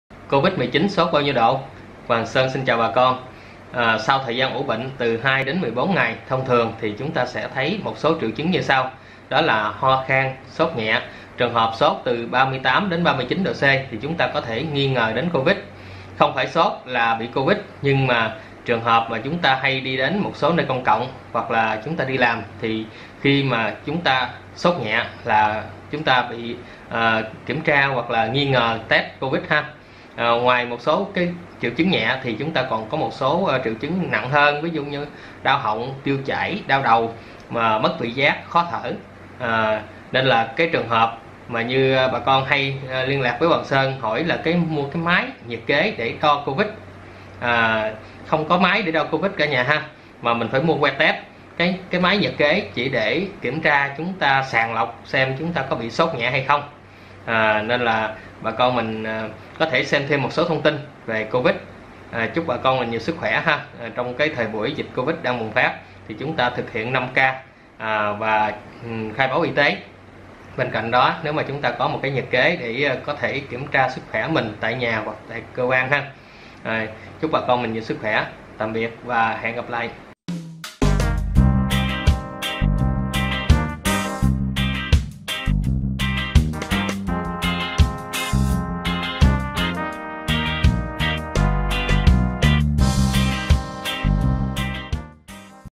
Covid 19 sốt bao nhiêu độ ? Sức khỏe 60s
Sau thời gian ủ bệnh (trong khoảng từ 2 đến 14 ngày),
 các triệu chứng nhiễm Covid 19 bắt đầu xuất hiện như
ho khan và sốt nhẹ.
Sốt được xác định nghi ngờ Covid 19 là từ 38,1oC – 39oC
Các triệu chứng thường gặp nhất:
sốt
ho khan
mệt mỏi
Các triệu chứng ít gặp hơn:
đau nhức
đau họng
tiêu chảy
đau đầu
mất vị giác hoặc khứu giác
Các triệu chứng nghiêm trọng:
khó thở
đau hoặc tức ngực
mất khả năng nói hoặc cử động
Nếu các triệu chứng càng kéo dài càng xuất hiện thêm nhiều triệu chứng tăng nặng, đòi hỏi người bệnh phải nhập viện, hồi sức tích cực và thở máy xâm lấn.
 Covid 19 có diễn tiến khó đoán, đặc biệt là ở bệnh nhân lớn tuổi có bệnh nền đi kèm